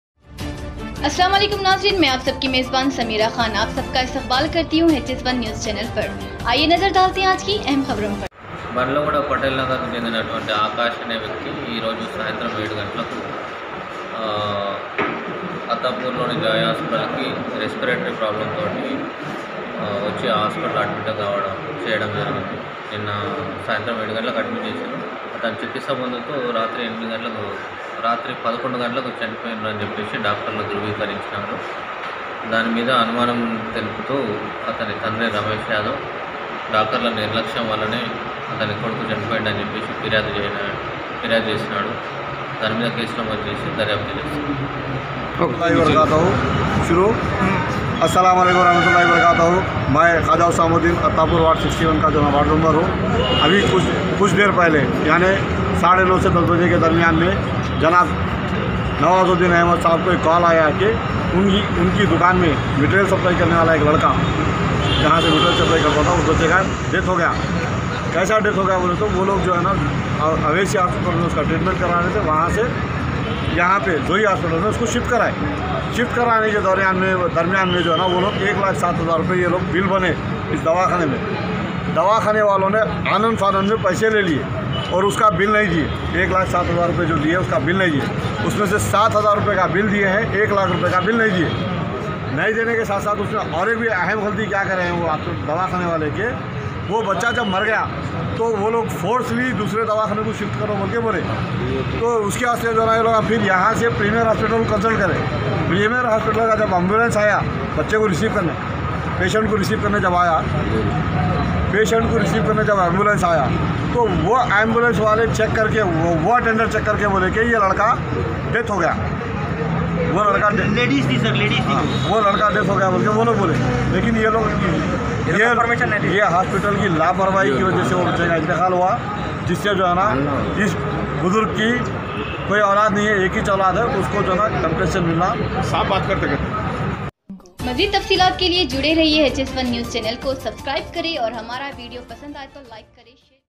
ZOI HOSPITAL ME 1NAWJAWAN KI MAUT PAR UNKE GHARWALON KA STAFF KE UPAR LAHPAWAHI KA ILZAM HS ONE NEWS
ZOI HOSPITAL K STAFF KI LAHPAWAHI SE EK NAUJAWAN KI HOWI MAUT KA ILZAAM

TAFASEELAT K MUTABIKH

BALLAGUDA  ME RAHENE WALE  AKASH NAAMI NAUJAWAN KO  ELAJ K LIYE OWASI HOSPITAL SE HAIDARGUDA ME MAUJOOD ZOI HOSPITAL ME ADMIT KIYA GAYA  THA  K ELAJ K DAUARAN STAFF K LAPARWAHI  SE AKASH KI MAUT  HOGAYI     JISKE  BAAD HOSPITAL  K PASS  AFRA TAFRI MACHGAYI

JAISE HI ISKI ETELA ATTAPUR OUTPOST K CI KARANTHI KO MILI FAURI MAUKHE PAR PANCHKAR HALAT KO KABO ME KARLIYA AUR EK CASE DARJ KARKE TAHQEEQAT SHURU KARDI. *@HS ONE NEWS HYDERABAD